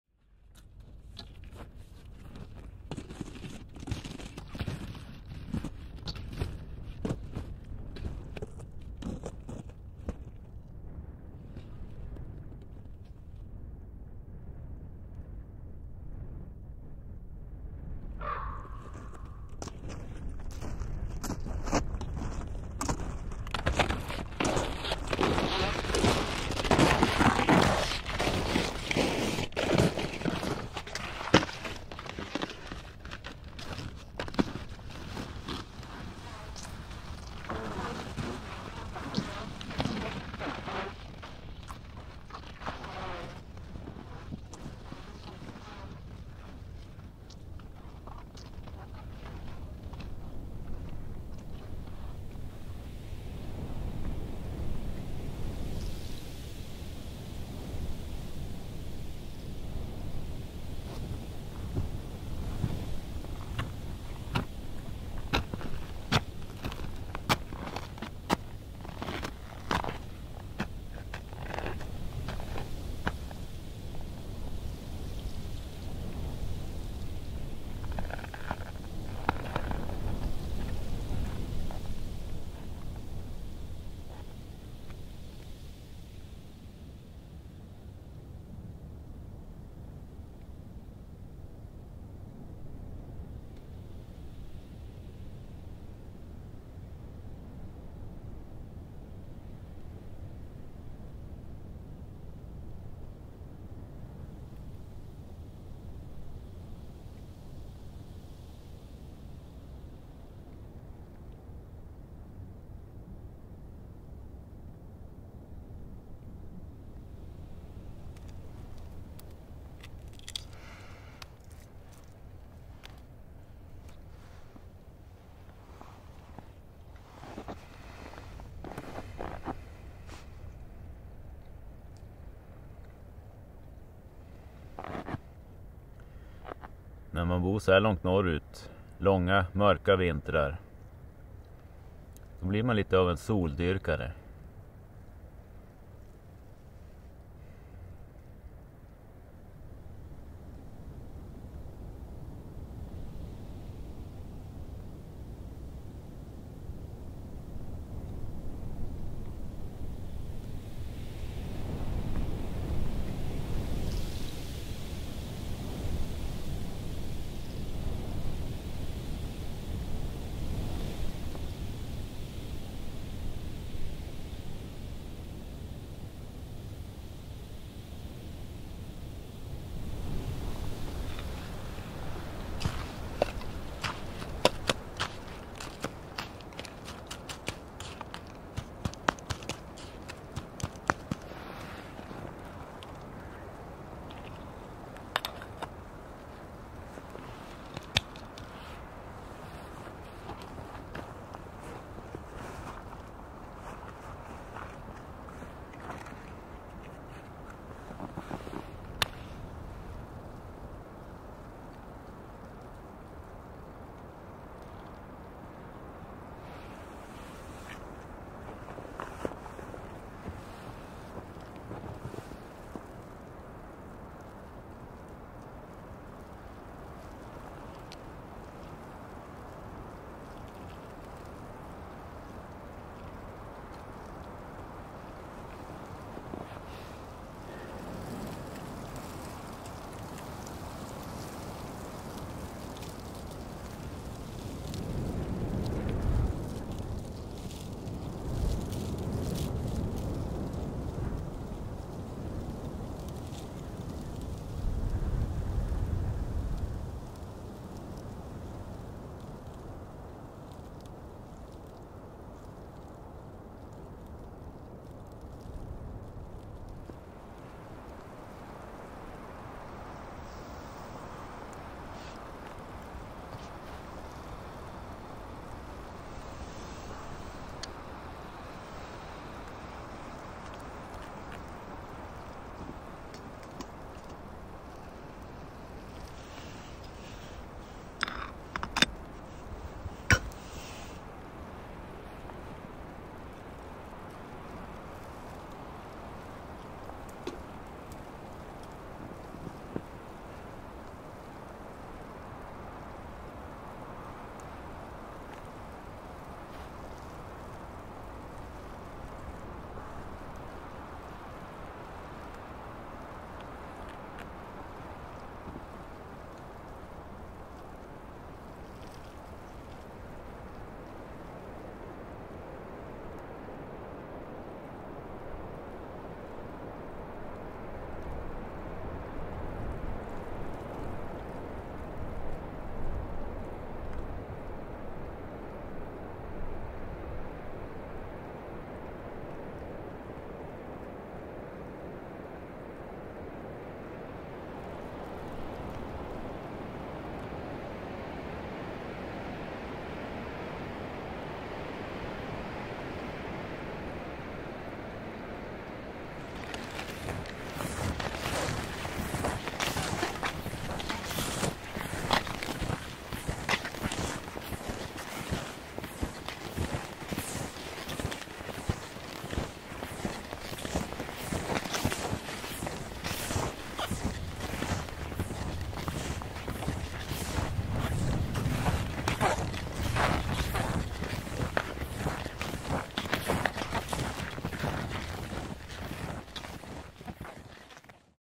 One Day in the Frozen World
Skiing on the ice of the Bothnian nay, Bottenviken, a cold day in January. The beauty of the endless white and frozen sea. Making coffee on the Trangia triangle.

Your support helps make more videos. Thank you!